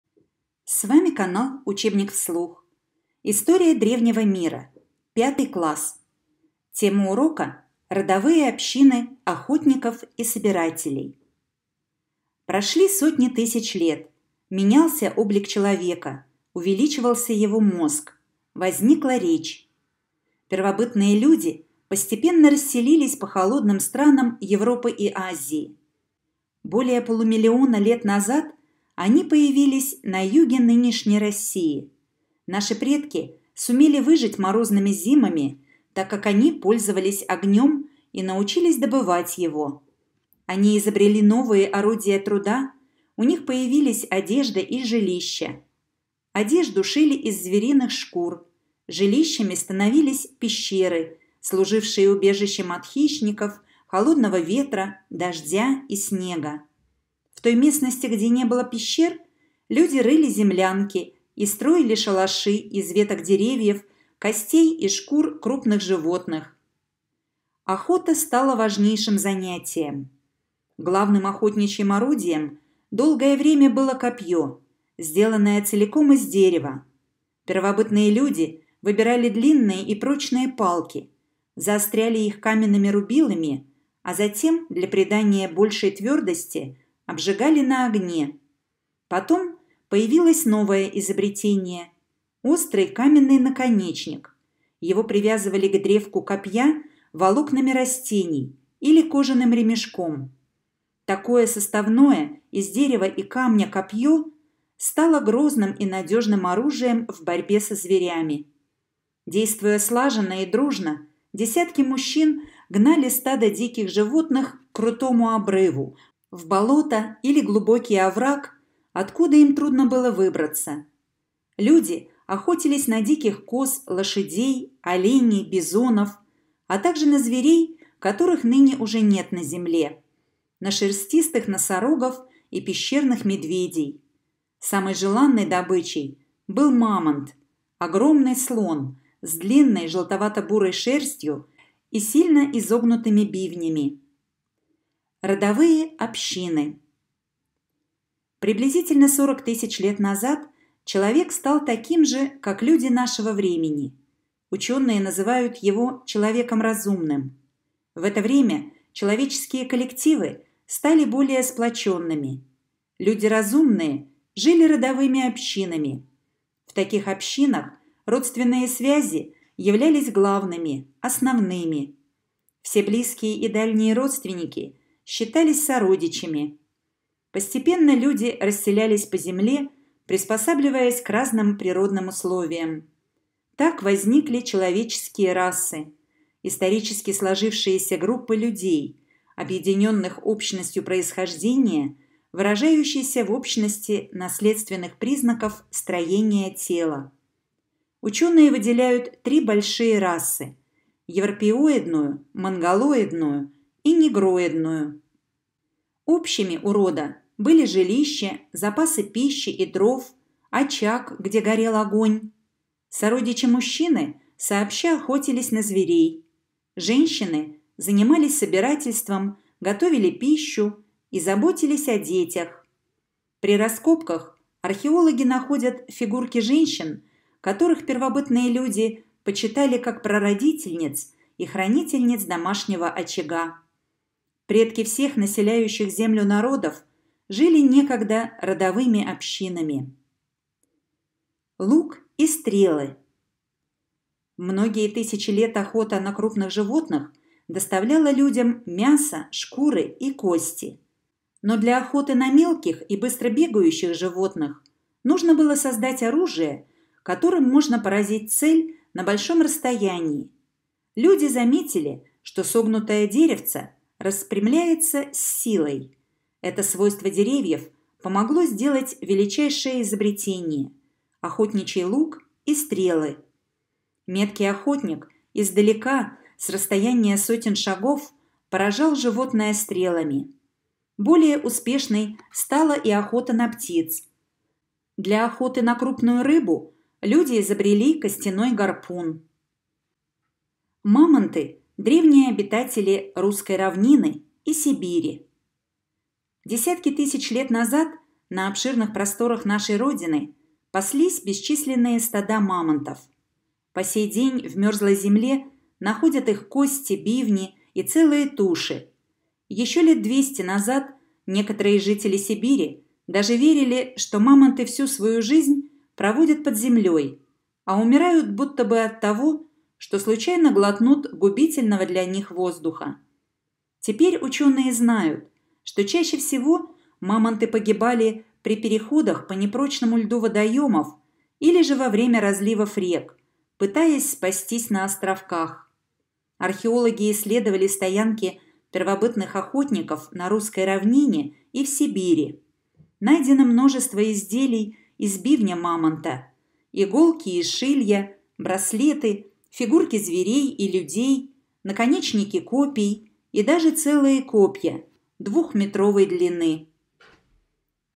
§2 "Родовые общины охотников и собирателей", История древнего мира 5 класс, Вигасин.
00:00 - Начало
00:25 - Первобытные люди постепенно расселились по холодным странам Европы и Азии
02:26 - Родовые общины
04:02 - Лук и стрелы
04:53 - *Мамонты - древние обитатели Русской равнины и Сибири

"Учебник вслух" - это параграфы, статьи и произведения из школьных учебников. Плейлисты по каждому конкретному учебнику! Уроки с комфортом!